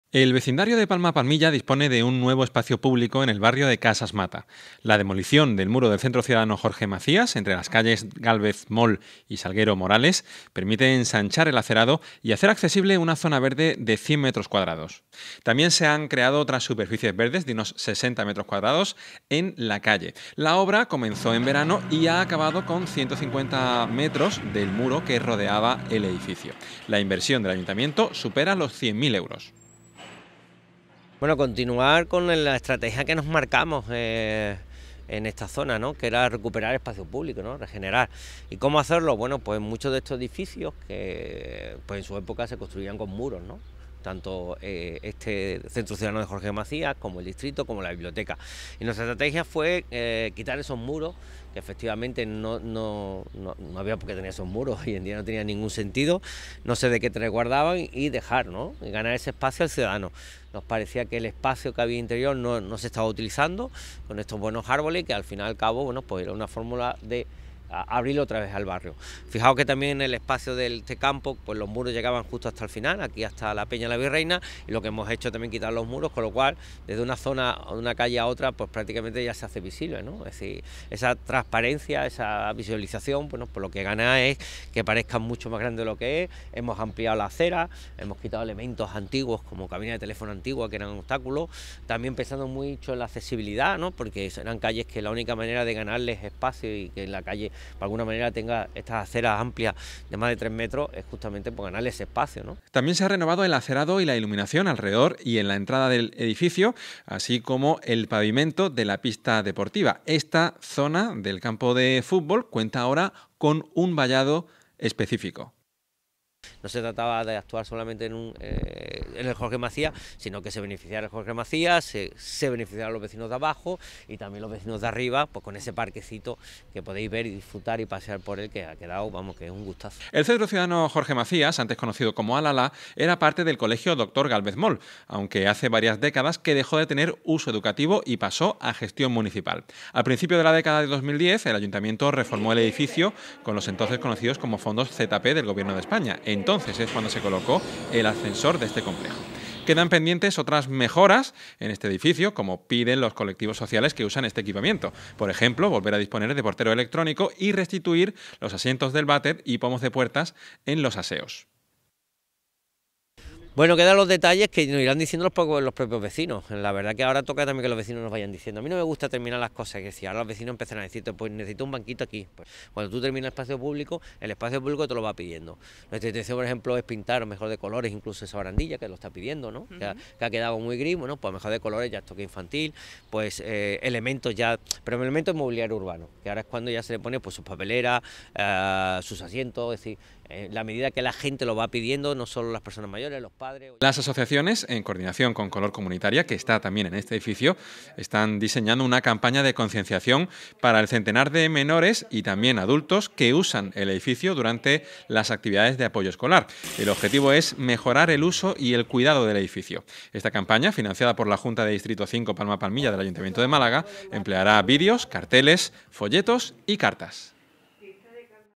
Remodelan el entorno de calles Gálvez Moll y Salguero tras tirar el muro del centro Jorge Macías
El vecindario de Palma Palmilla dispone de un nuevo espacio público en el barrio de Casas Mata. La demolición del muro del centro ciudadano Jorge Macías, entre las calles Gálvez Moll y Salguero Morales, permite ensanchar el acerado y hacer accesible una zona verde de 100 metros cuadrados. También se han creado otras superficies verdes de unos 60 metros cuadrados en la calle. La obra comenzó en verano y ha acabado con 150 metros del muro que rodeaba al edificio. La inversión del Ayuntamiento supera los 100.000 euros.